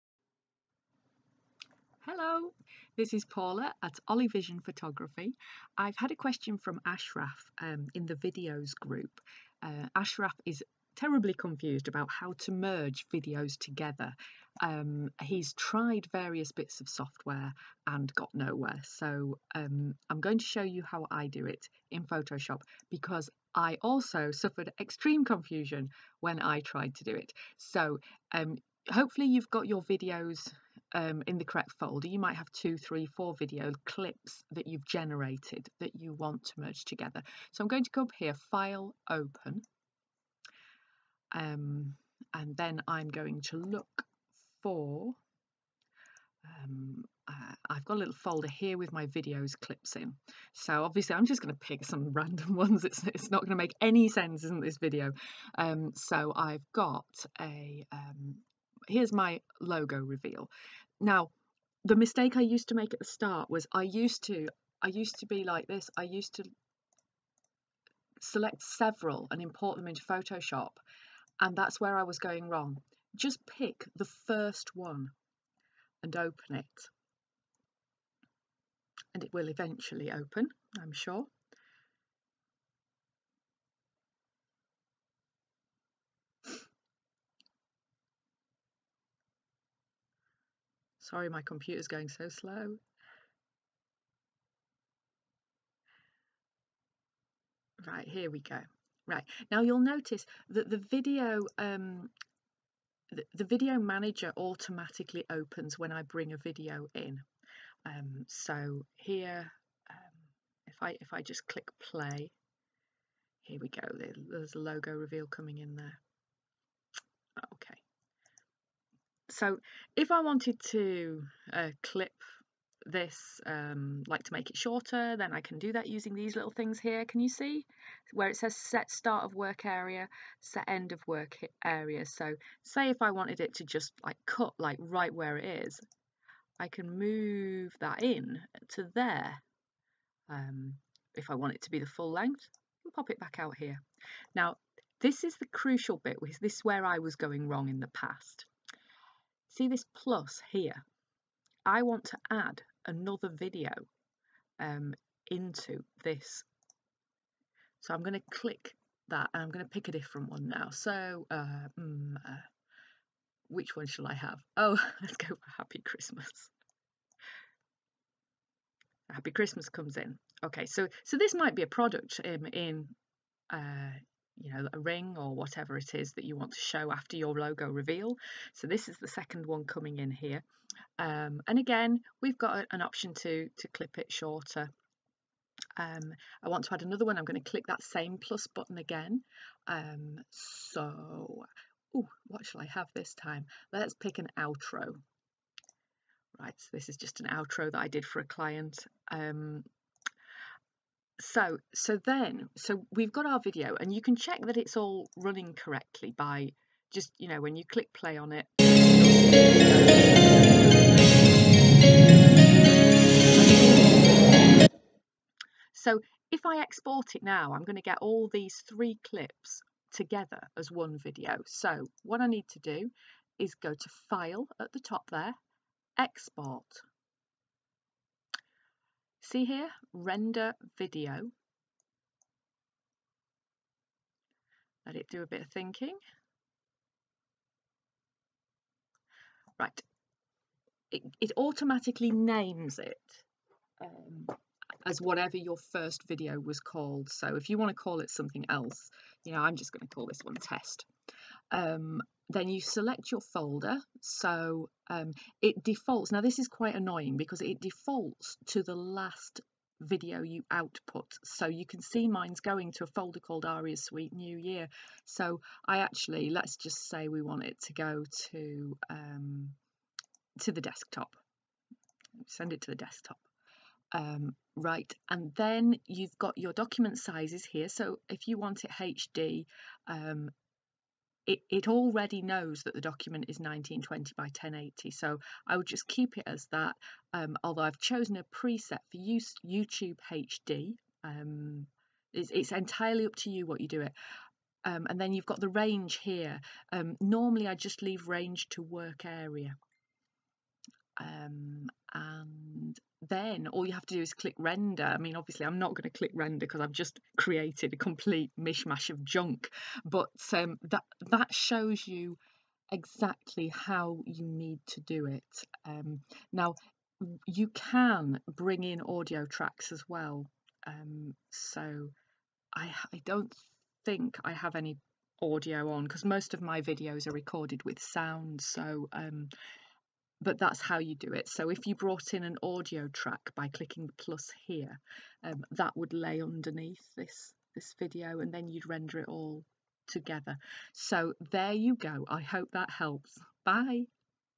How to join videos together in Photoshop CC
A very simple walk through of how to stitch video clips together in photoshop.
This is intended for absolute beginners!